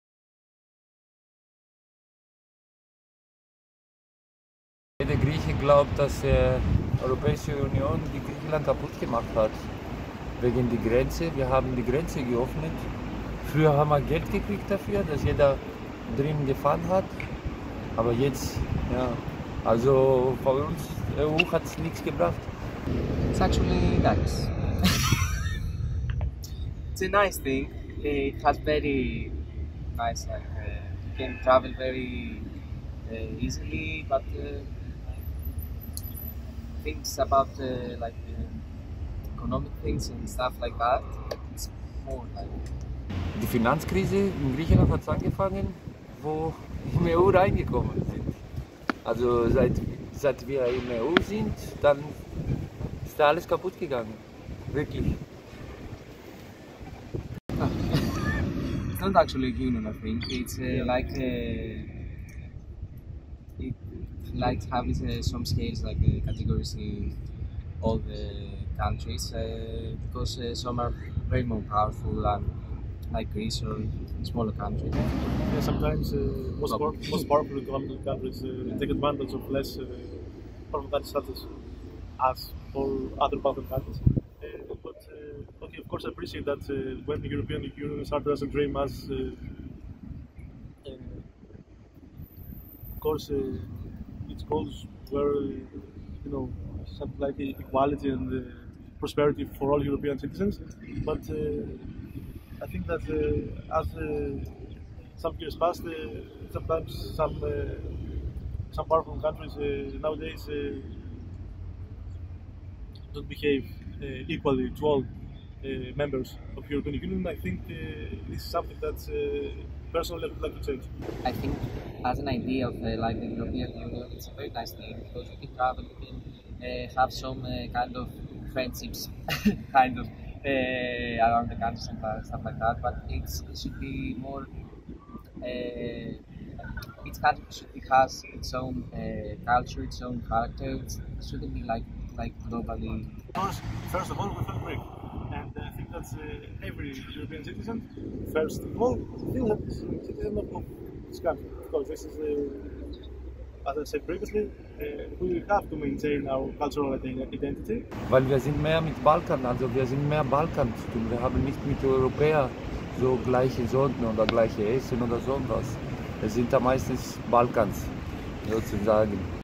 Darum sind die Griechen EU-skeptisch
Umfrage in Thessaloniki: Migration, Finanzkrise, ungleiche Machtverteilung – darum kritisieren viele Griechen die EU. Die Grundidee und einfaches Reisen finden die meisten aber positiv.